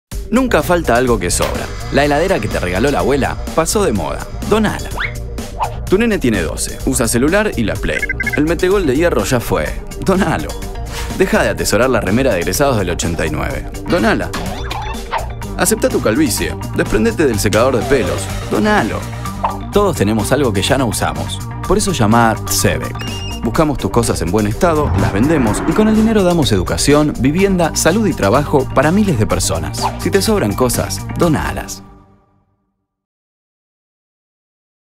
Tzédek - el emprendimiento solidario de la Fundación Tzedaká
Muchos ya colaboraron donado cosas que ya no usan. Te invitamos a que vos también colabores llamando al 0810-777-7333.